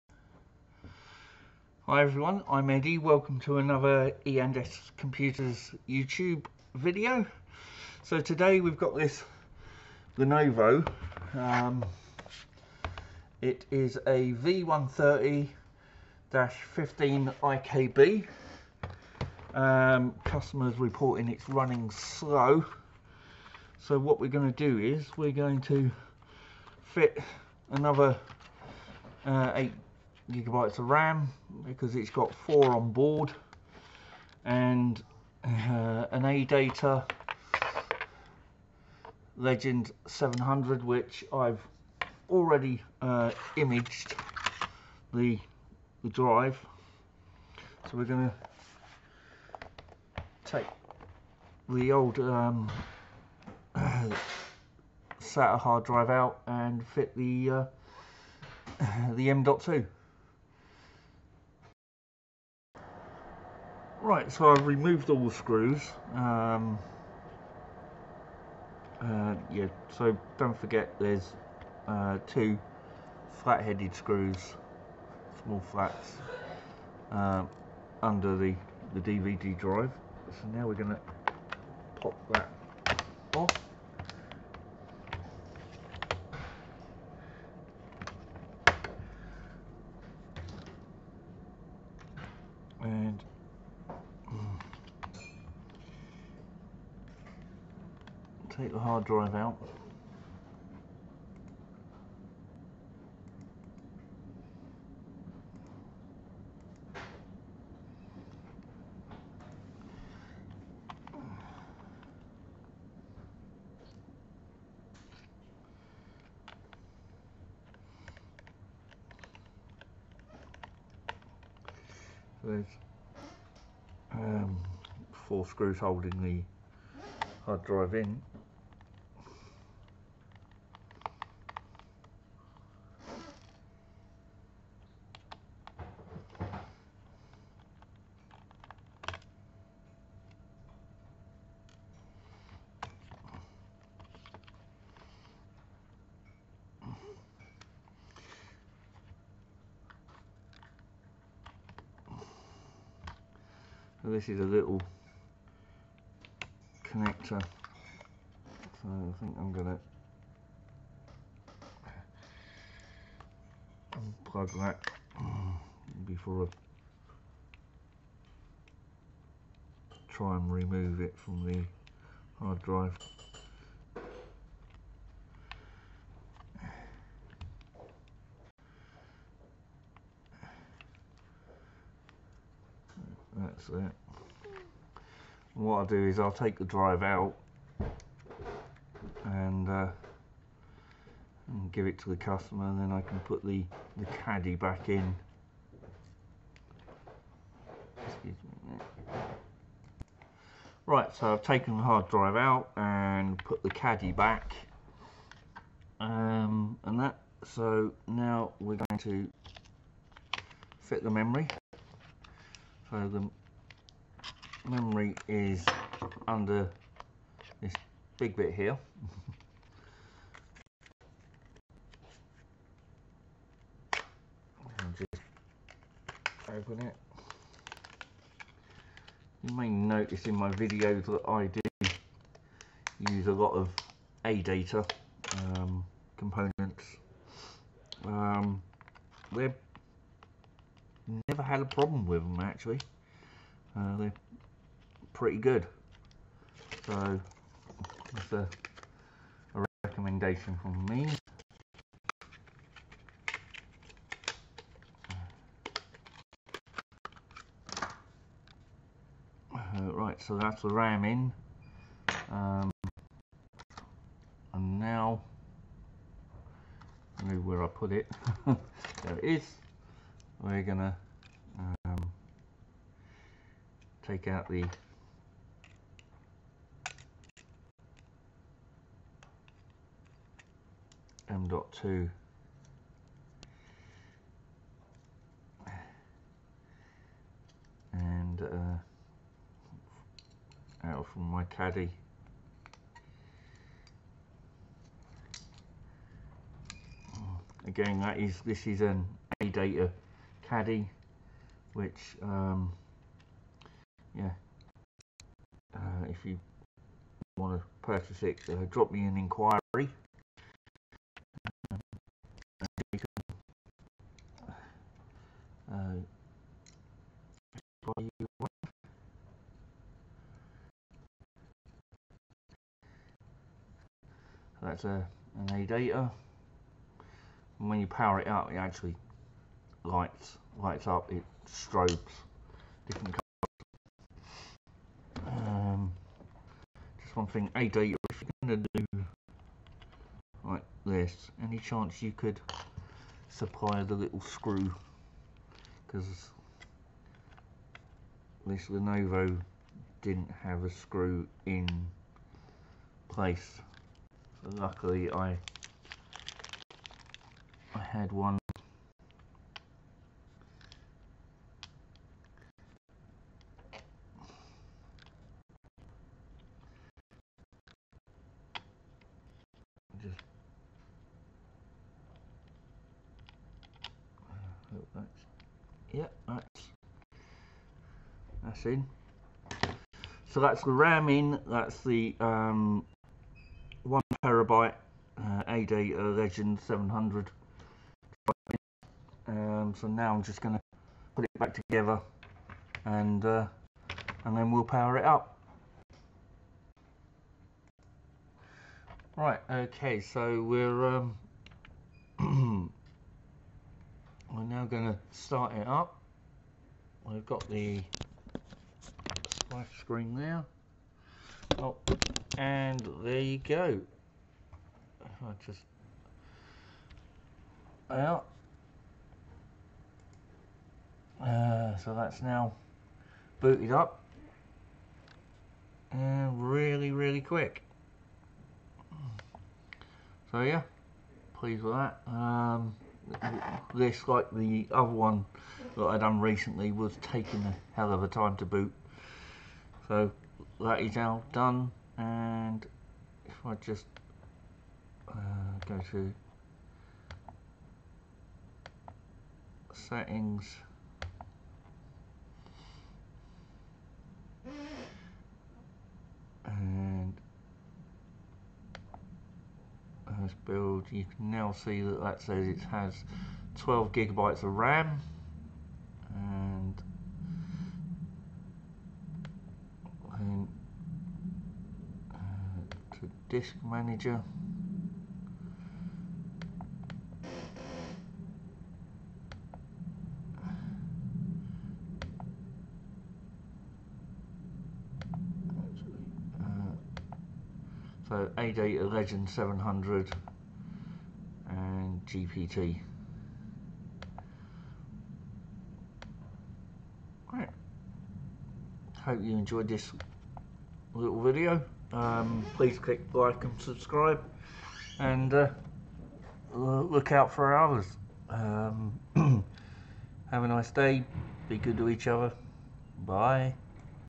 How to install a RAM MODULE & M.2 SSD into a Lenovo V130-15 in under 10 minutes!!!
Customer complaining laptop is running slow and requested adding 8GB to the 4GB onboard RAM and replacing the Western Digital HDD with a ADATA Legend 700 M.2 SSD on their Lenovo V130.
Instead of doing a fresh install I cloned the HDD onto the M.2 SSD

E & S Computers Information:
Address: Unit 27, Ilton Business Park, Ilton, Ilminster TA19 9DU